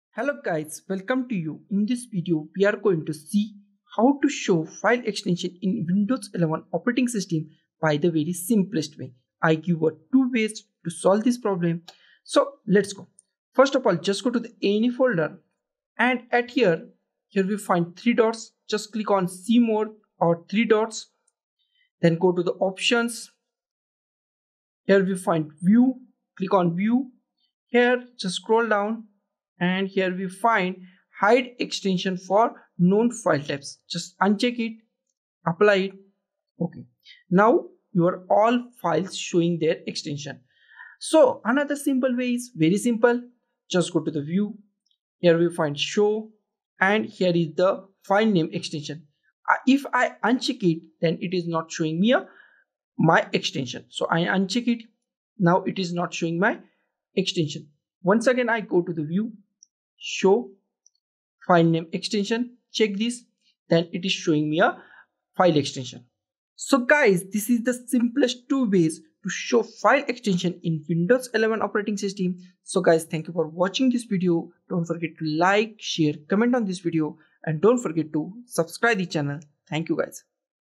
How to Show File Extensions in Windows 11 PC - Laptop
In this video we are going to show How to Show File Extensions in Windows 11 PC - Laptop by the two simplest ways .